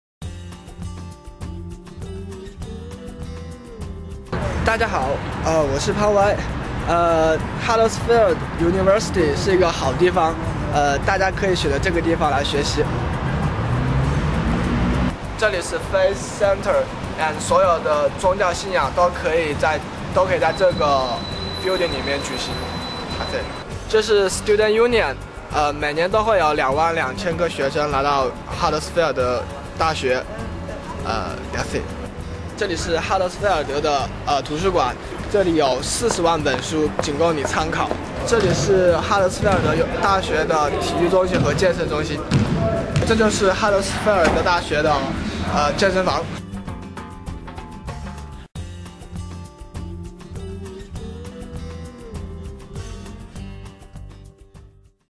University of Huddersfield campus tour with Will (Chinese)
International Study Centre student, Will,  goes on a quick tour of the University of Huddersfield campus.